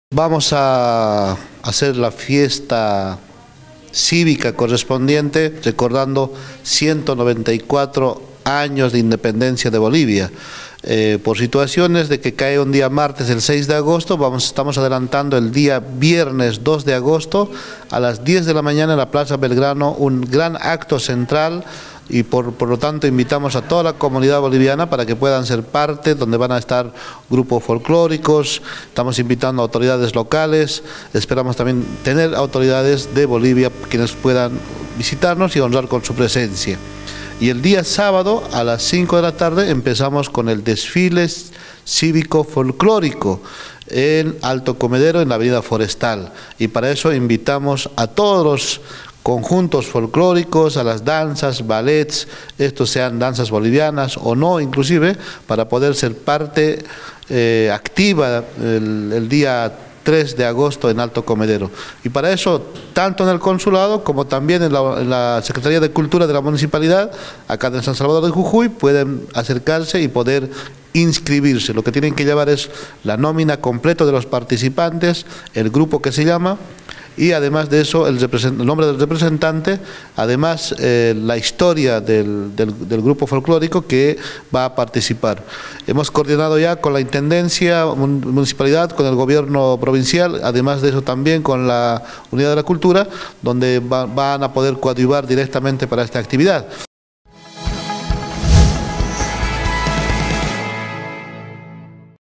Se viene un nuevo aniversario de la independencia de Bolivia y habrá festejos en Jujuy
Habrá un desfile en la capital jujeña por el 194 aniversario de la independencia del vecino país. ¿Vas a ir?